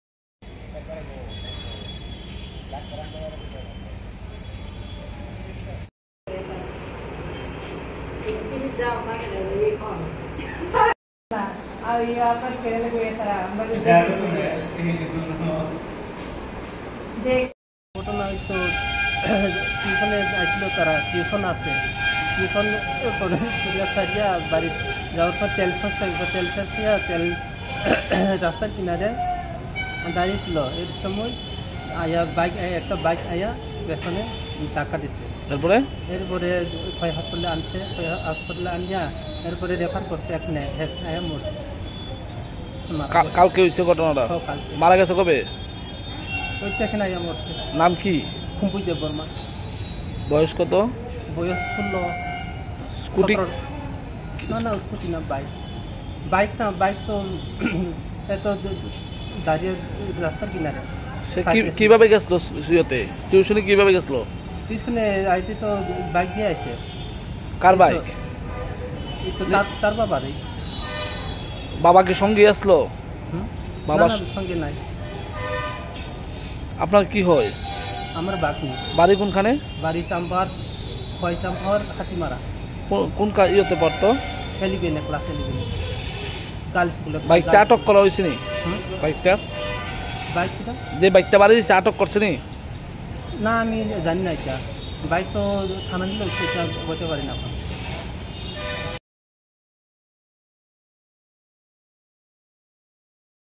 বাড়ি যাওয়ার পথে বাইকের ধাক্কায় মৃত্যু এক ছাত্রীর নাম খুম্পুই দেববর্মা(১৬)ঘটনা খোয়াই এলাকায়
টিউশন থেকে বাড়ি যাওয়ার পথে বাইকের ধাক্কায় মৃত্যু এক ছাত্রীর।নাম খুম্পুই দেববর্মা (১৬) ঘটনা খোয়াই এলাকায়।।।।।।